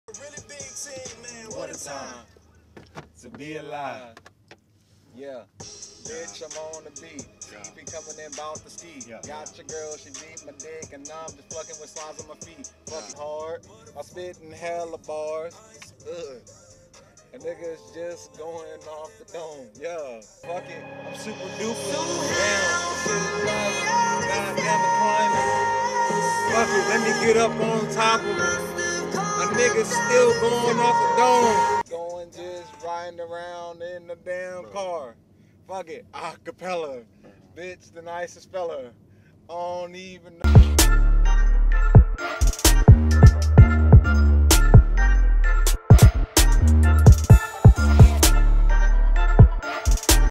When your friends always freestyling
When Your friends stay freestyling! Lol i cant be the only one that experienced this, bro at first its cool i kind of vibe with homie but then he always start feeling himself. And thats cause for disaster right there because now no matter whats playing he feel like he got the juice so he raps over it lol TAG your friends that do this